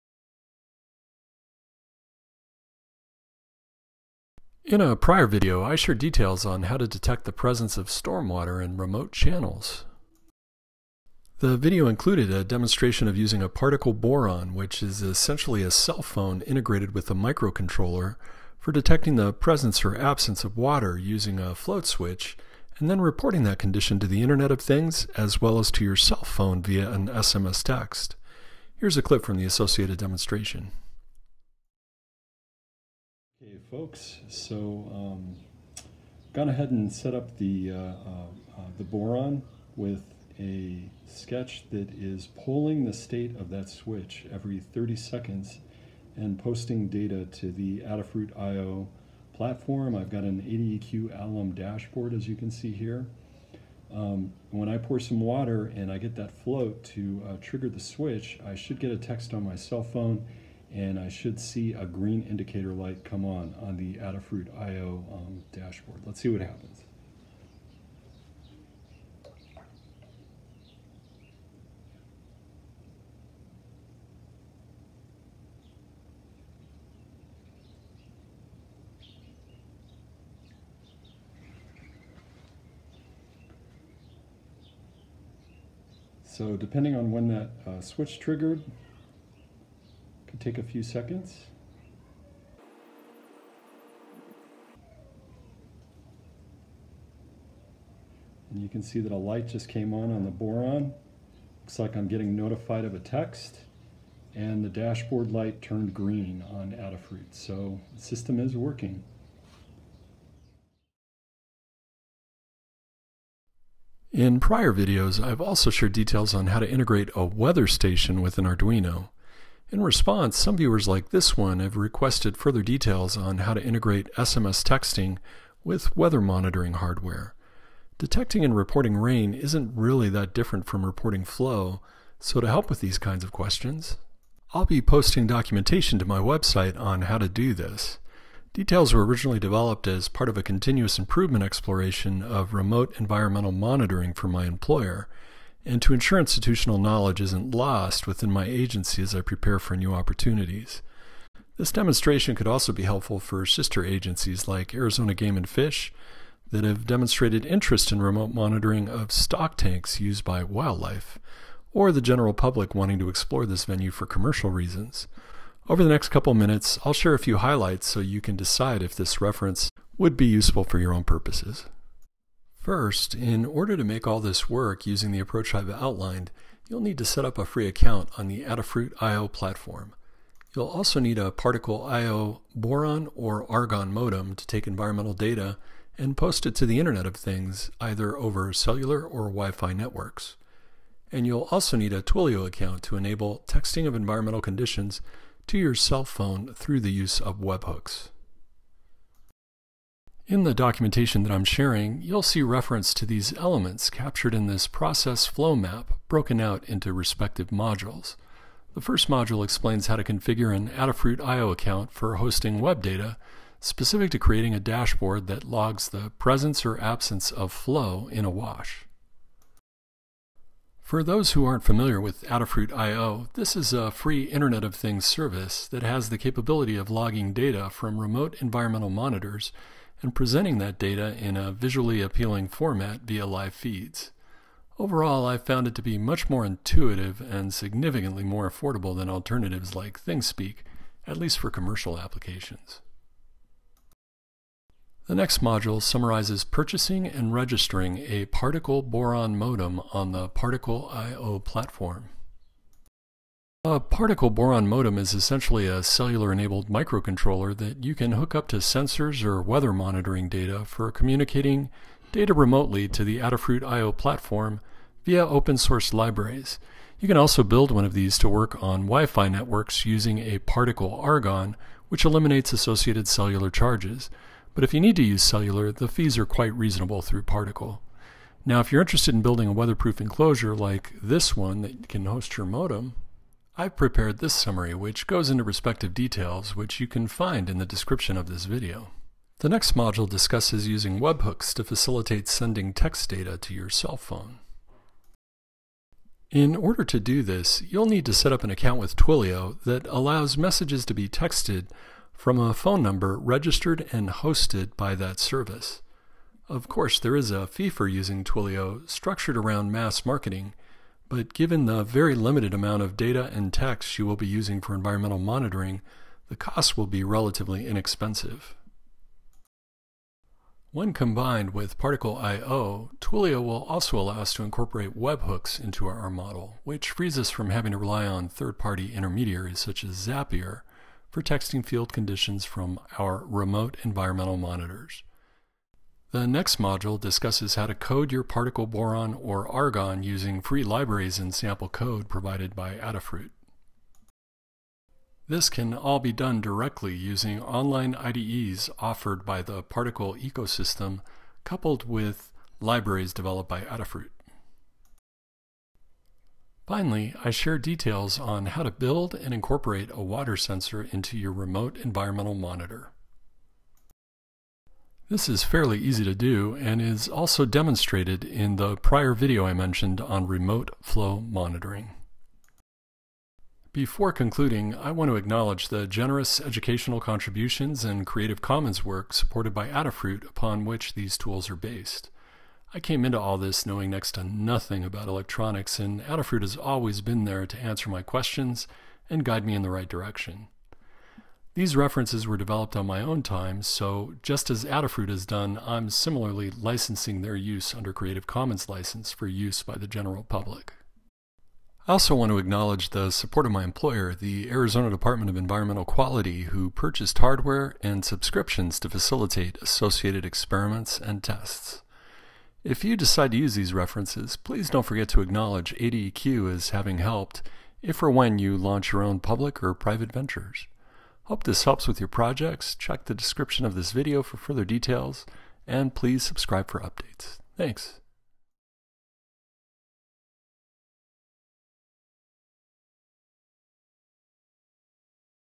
4G SMS Text and IOT With Particle IO
This video summarizes how to set up a Particle Boron modem with Twillio and Adafruit IO for (1) posting data to the Internet of Things and (2) sending SMS texts to your cell phone when something interesting happens in the environment (e.g. when stormwater is detected).

If you find this useful, you can download the referenced documents
(Scroll down to the bottom of the page for the archive under Chapter 32.)

All documentation is licensed under Creative Commons - feel free to copy and use if it helps with your own projects, but please acknowledge Hans Huth and ADEQ if you so so.